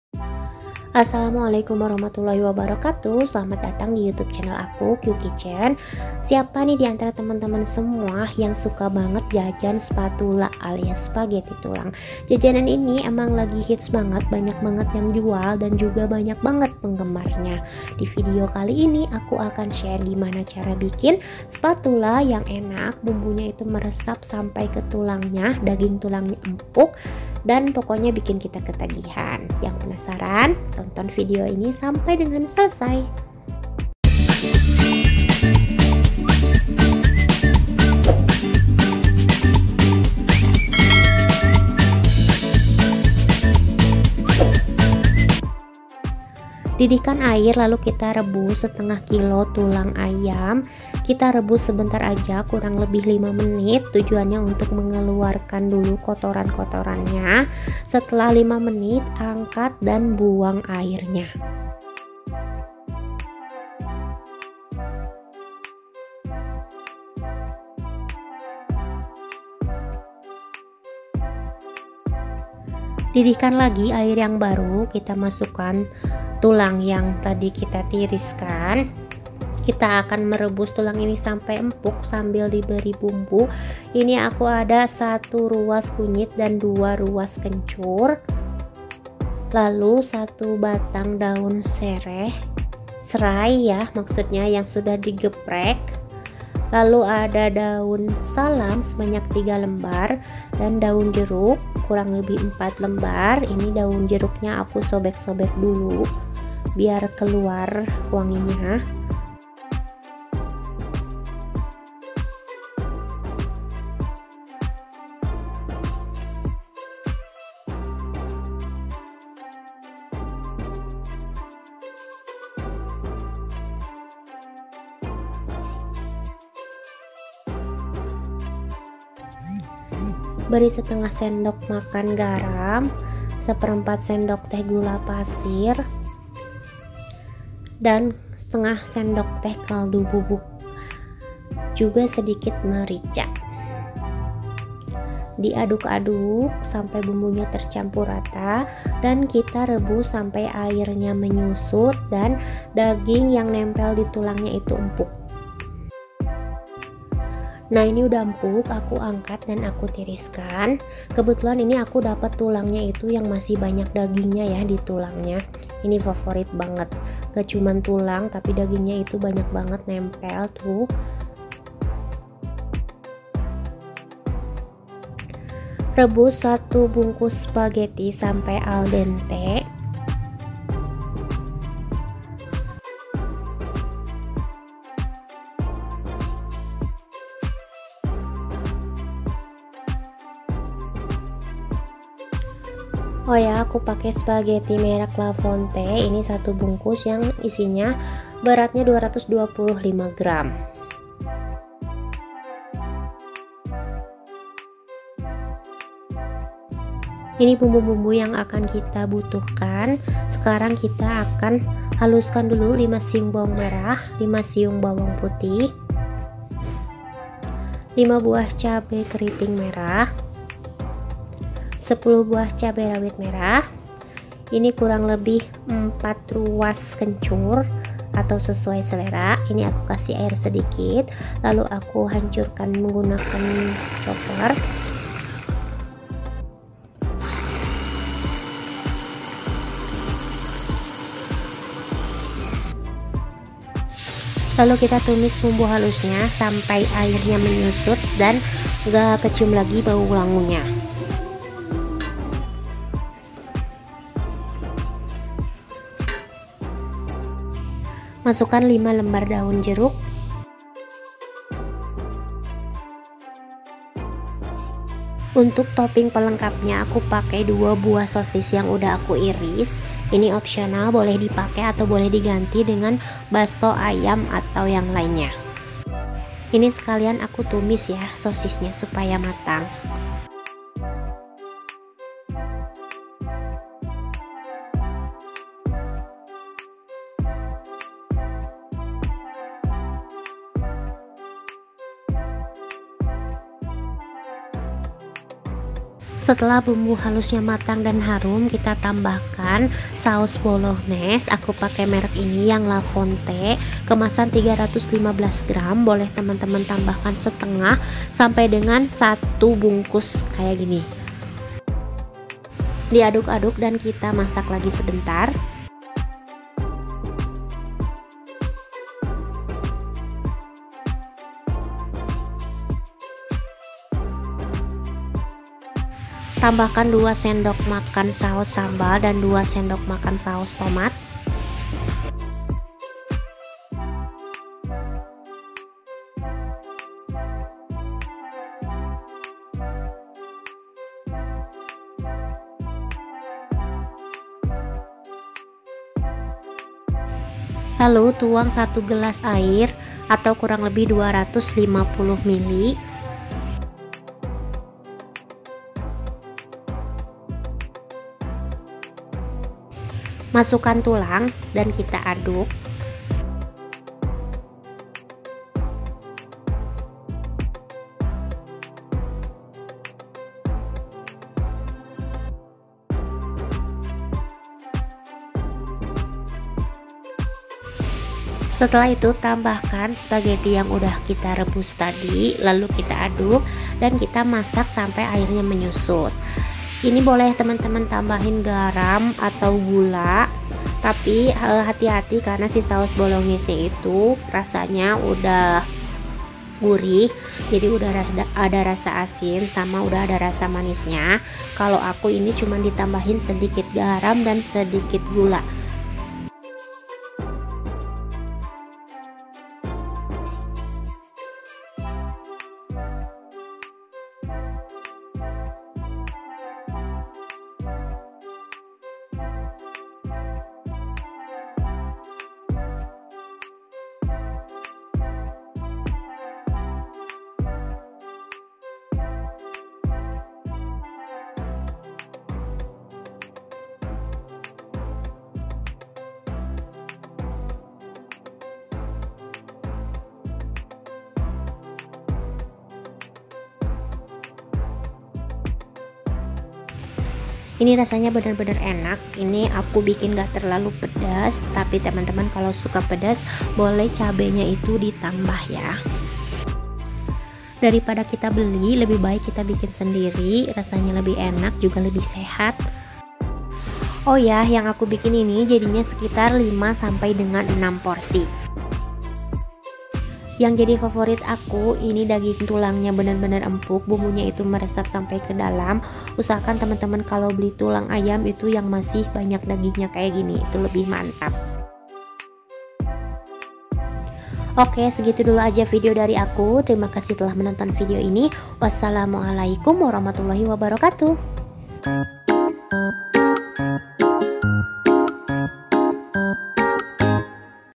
gak akan jajan spatula di luar lagi kalau udah tau resep ini ! || spaghetti tulang bumbu mantap
kalau jajan spatula kadang suka gak pas gak sih rasanya? nah kalau bikin sendiri kan lebih enak dan lebih higienis. bisa juga buat ide jualan :)

SPAGHETTI TULANG

500 gr tulang ayam
1 bgks spaghetti la fonte

bumbu utk merebus tulang :

1 ruas kunyit, haluskan
2 ruas kencur, haluskan
1/2 sdm garam
1/4 sdt gula
1/2 sdm kaldu bubuk

bumbu spaghetti :

5 siung bwg merah
5 siung bwg putih
10 bh cabe merah keriting
20 bh cabe rawit merah
4 lembar daun salam
4 lembar daun jeruk
1/2 -1 bgks saos bolognese
2 sdm saos sambal
2 sdm saos tomat
garam, gula (kalau perlu)

topping pelengkap:
2 bh sosis

bisa jadi 5-6 porsi